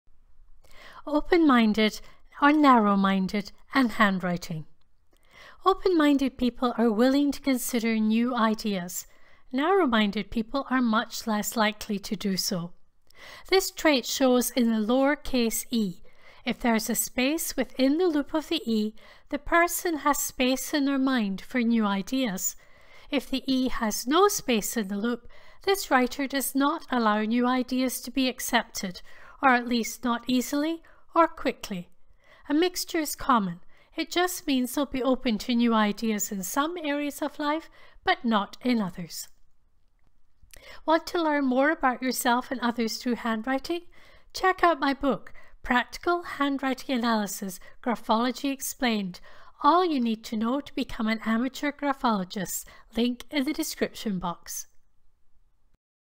Open minded/narrow minded from Handwriting
Open minded people are willing to consider new ideas. Narrow minded people are much less likely to do so. Find out how to identify this personality trait in any Handwriting.
My book “Practical Handwriting Analysis: Graphology Explained – all you need to become an amateur Graphologist” is on Amazon.